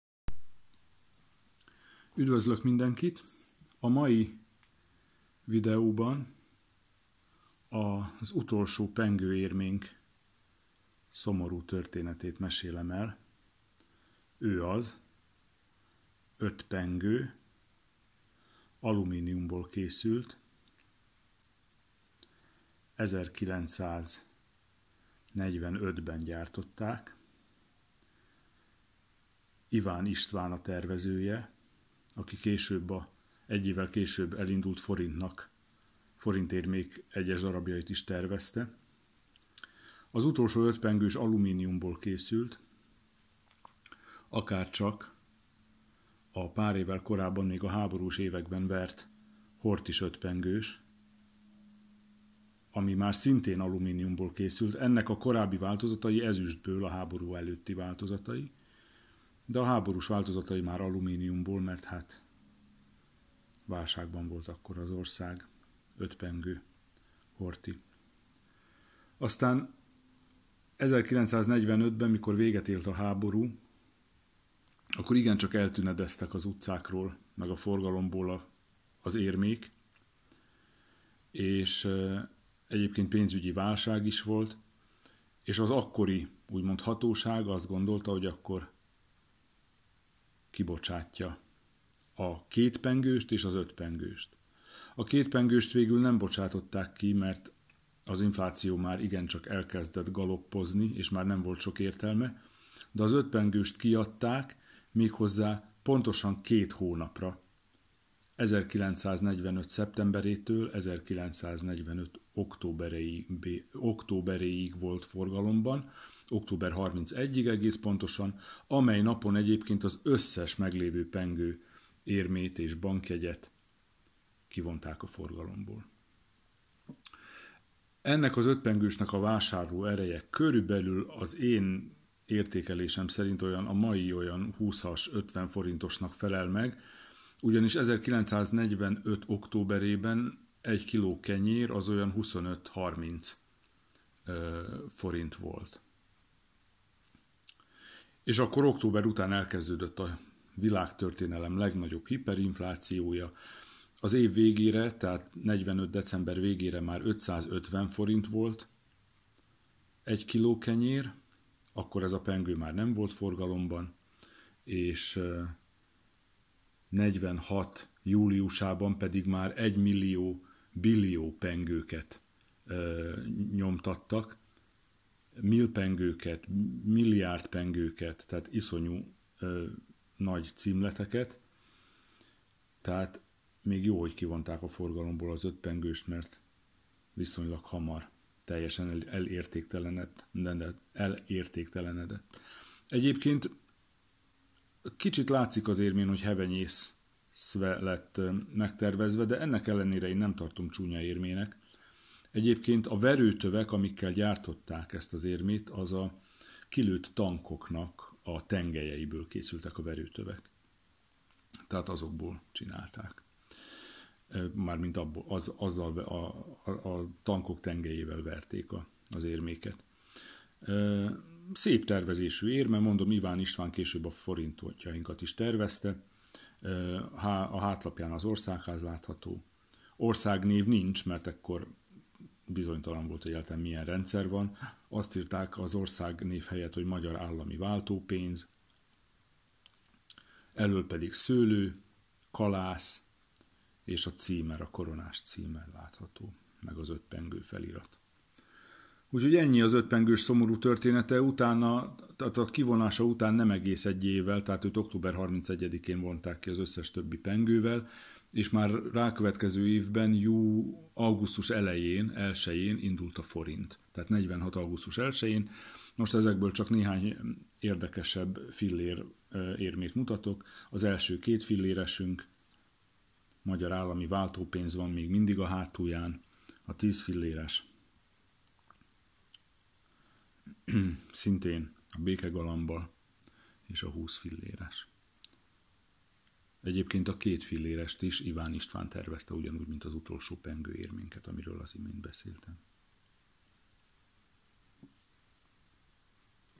Az utolsó pengő érme szomorú története - Érmebemutató 6.
Az 1945-ben vert 5-pengős alumínium érme mindössze 2 hónapig volt forgalomban. Elsöpörte a világtörténelem legnagyobb hiperinflációja, utána meg bevezették a forintot.